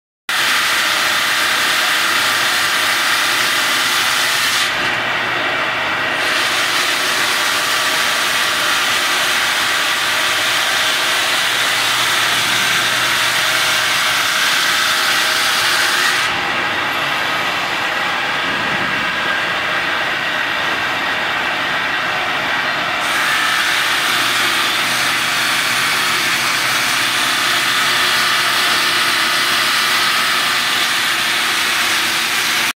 JOBORN——Automatic Profiling Machine 1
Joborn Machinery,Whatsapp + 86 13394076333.
This machine automatically track model of curve(straight) line through the fiber optic probe, automatically profiling through diamond circular saws to cut stone with curve(straight) line and craft products, uses inverter to control left-right feeding, adjusts the speed according to the stone material. If choosing rotating worktable, the processing of arc-shaped lines can be done. This machine adopts microcomputer control system, high degree of automation, high precision cutting; using double-blades designing, with high cutting efficiency, high stability, easy-operation to suit for lines, arc-shaped lines, Roman column, etc.
Note: can modify with CNC system, which removes from the trouble of making the templates, with high automation and high cutting precision.